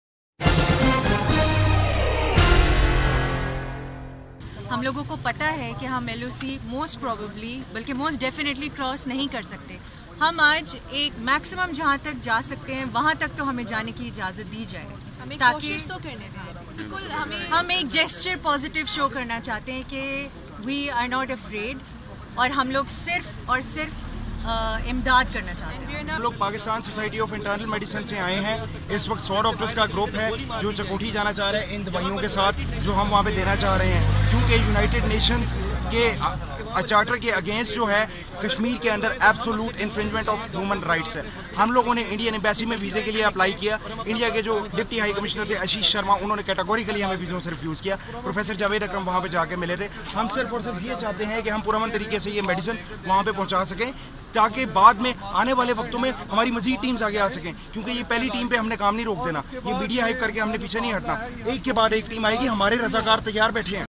Pakistani Doctors march to Line of control Chakothi
Largest media group of AJK